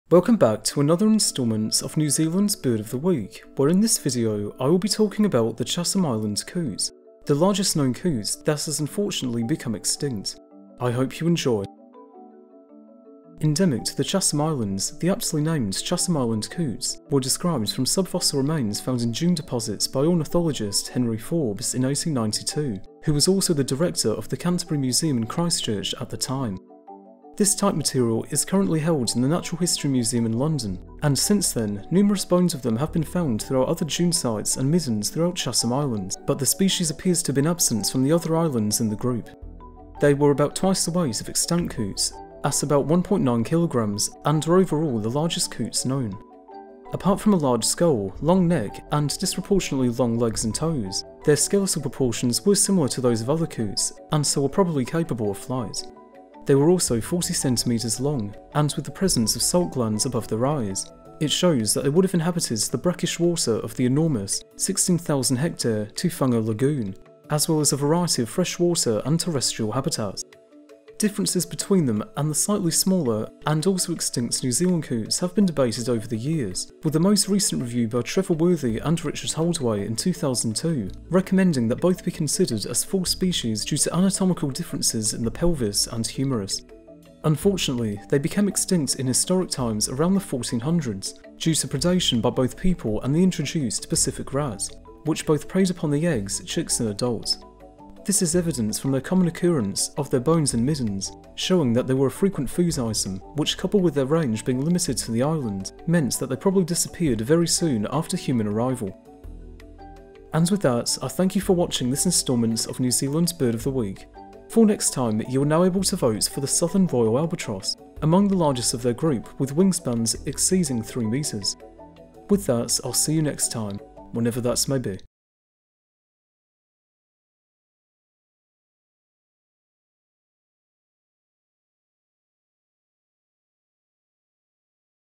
Chatham Island coot - New Zealand Bird of the Week
Chatham Island coots were the largest known coots that have unfortunately become extinct, being found on and named after Chatham Island where they were discovered and described by ornithologist Henry Forbes. I hope you enjoy.